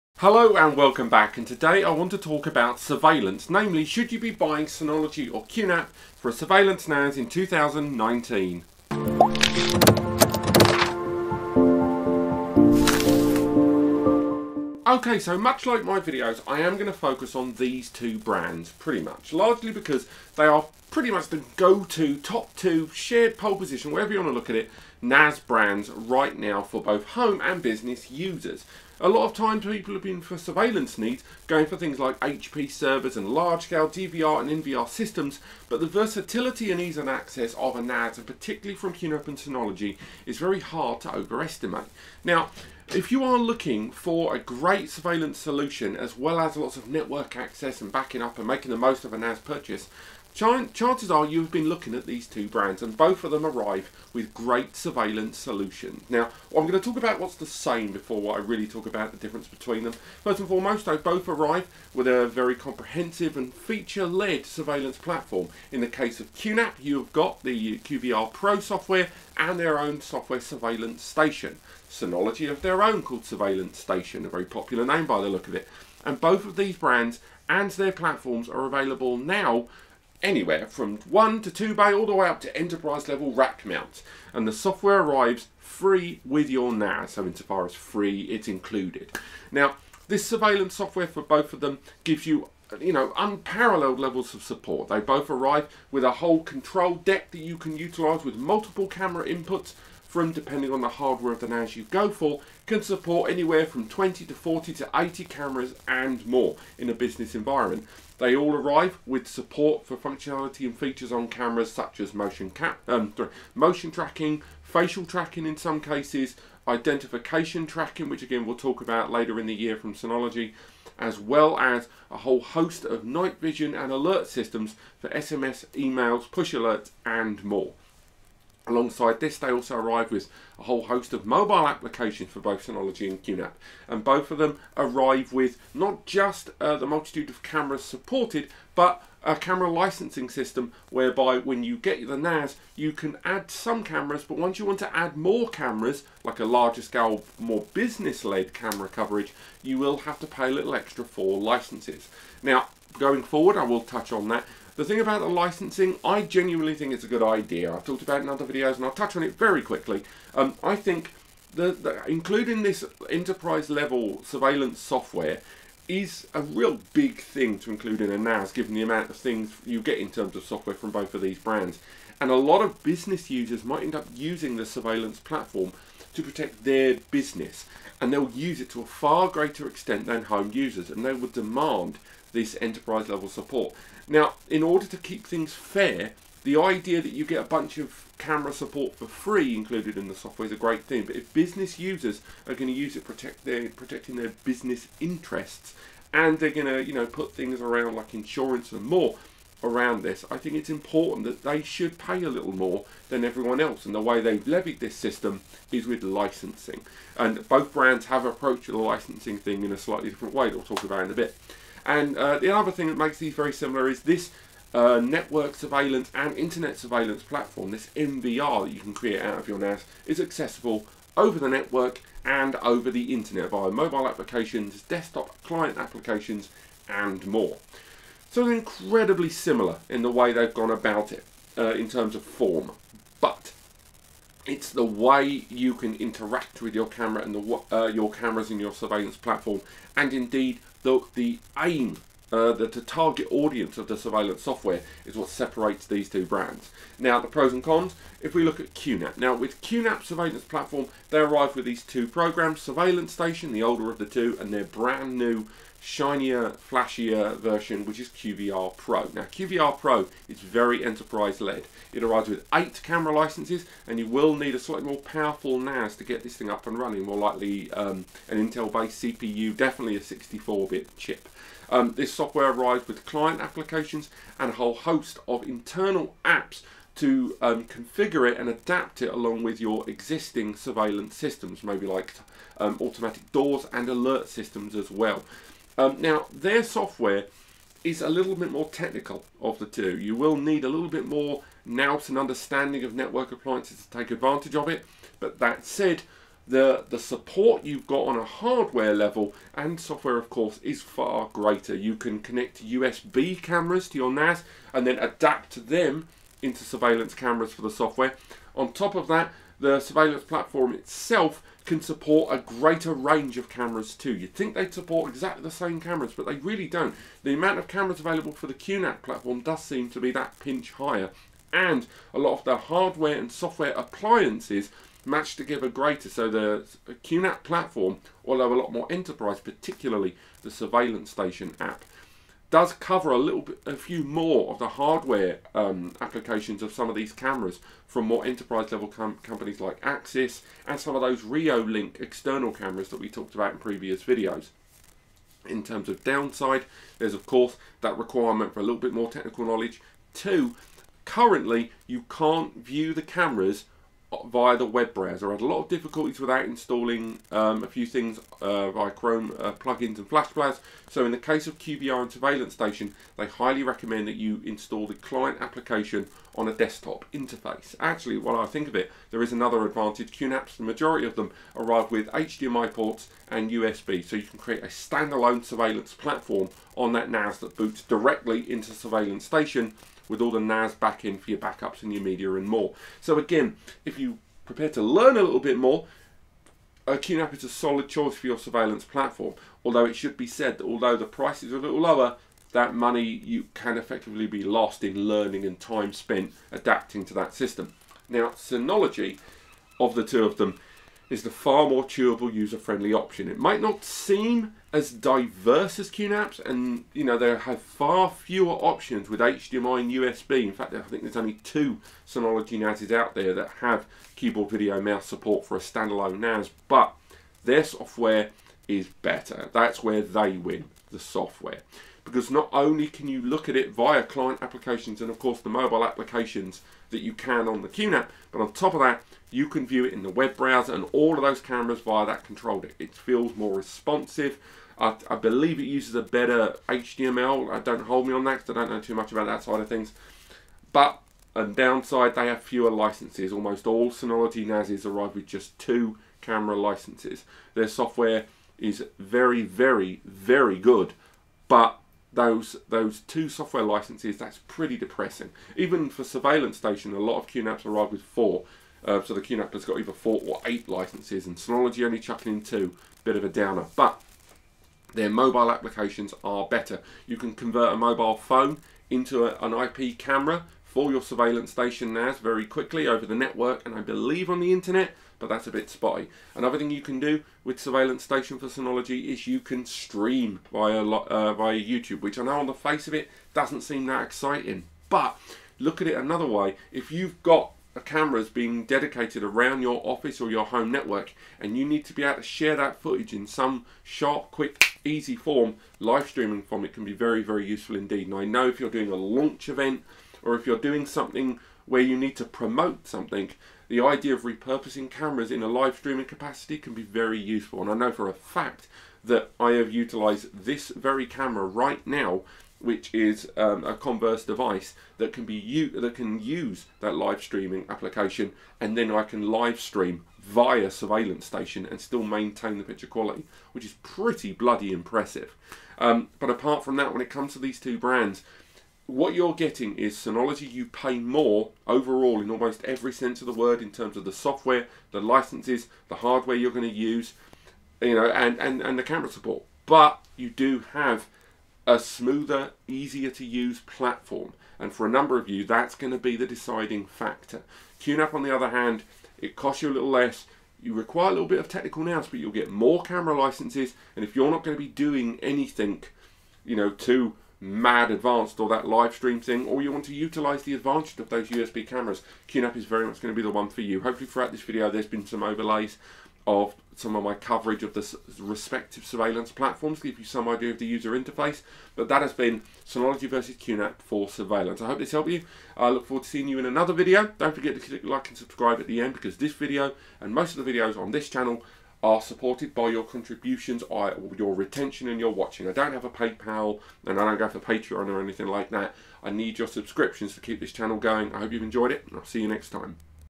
Synology Vs QNAP for Surveillance in 2019
This description contains links to Amazon. These links will take you to some of the products mentioned in today's video. Things to consider when you buy a Synology or QNAP NAS this year

When you buy your first network attached storage device, it can be an incredibly daunting task. If you have a less technical background, or heard about NAS servers from Synology or QNAP, but are still not quite certain what a NAS is or what to look for in choosing the right device, it can be very intimidating. Luckily the learning curve is not as high as you think and both Synology and QNAP have really streamlined their NAS range, along with improvements in accessibility and user-interface over the years.

However for many NAS novice buyers, this is still not enough and with hundreds of Synology NAS disk stations and QNAP Turbo NAS devices to choose from, you can get a little lost. No one wants to get it wrong the first time and spend the next few years stuck with a device that does not live up to their expectations or needs. So, rather than going around in circles, why not use the following recommended factors when buying your first NAS server from Synology or QNAP. Below I have detailed the most common factors and choices people have to make when deciding on the best NAS to buy.

Choosing the Right NAS operating System that suits you – DSM Versus QTS
The Primary operation system of Synology NAS device is Diskstation Manager (or DSM) and it will be your User Interface for most interactions with your NAS server (except for when you use 3rd party applications like Plex, Apple Time Machine, DLNA streaming devices, etc). It has evolved incredibly over the years from a very basic file-level access platform to a fully featured operating system that pretty much enables you to do ANYTHING a PC or Mac Operating system can do. A demo of the software is available HERE. At the time of writing, it is in DSM 6.1 with 6.2 in beta. Many will liken the software to a more Mac-friendly platform. WIth fewer apps available in the app store and most of the technical information hidden a few layers deeper in the software, it is designed for those with less technical knowledge but want a stable platform to work with and a stronger first-party app foundation to work with.

Synology DSM Desktop QNAP QTS Desktop
Likewise, QNAP has spent years refining their NAS operating system with QNAP Turbo Station, or QTS. It shares many of the features and functionality of the Synology counterpart, but whereas the Synology DSM software hides all the technical information and restricts actions to a more stable and controlled environment, QNAP and QTS given you the technical information far more readily. Also, you can configure and tweak the QNAP far, far more in terms of productivity and individuality. Finally, there are far more 3rd-party apps available with the QNAP software, meaning you can install NAS apps officially and unofficially, such as Kodi, Netflix and more with the QNAP homebrew and community QPKG development forums. Whereas Synology DSM is more about iOS design, QNAP and QTS are far more in the vein of Android and windows. If you ever built a PC, or use an Android device for its flexibility and individuality, then you will certainly vibe with the QNAP  NAS software more. You can try a demo of QNAP QTS below: